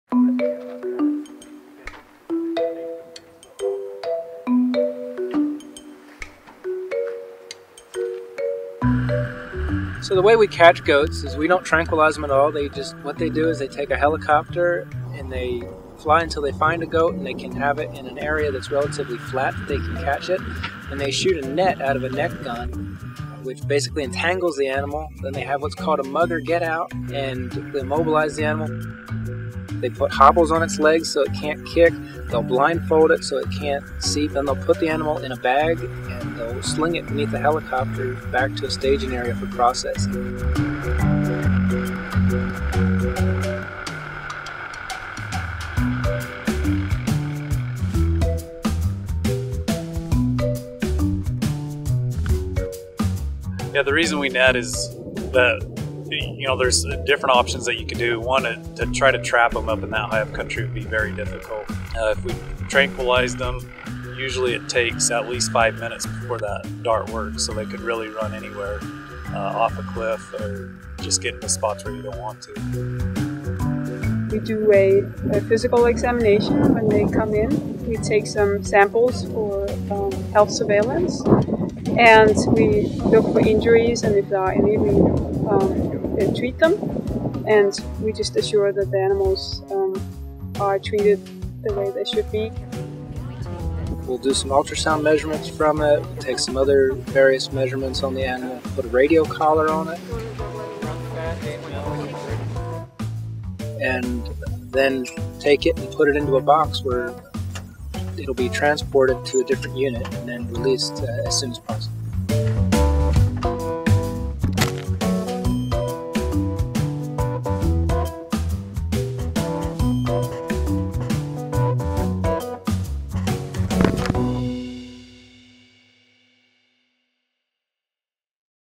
The Best Way to Move Mountain Goats? Helicopters. | National Geographic
How do you move a mountain goat nearly a hundred miles? With a helicopter. Mountain goats were introduced to Utah in the late 1960s, and since the initial release of six goats, their population has increased to more than 2,000. When a group gets too large for its range, the Utah Division of Wildlife Resources captures and moves some of the goats to other populations throughout the state. The best way to capture the goats is to shoot them with a net gun, strap them in a harness, and then move them by helicopter. The process is mostly harmless to the goats, though they may suffer small cuts when tangled in the net. Netting is safer for the goats than using tranquilizers, as a partially tranquilized goat could be hurt even more. A veterinarian is on hand to make sure they're healthy and safe before being released.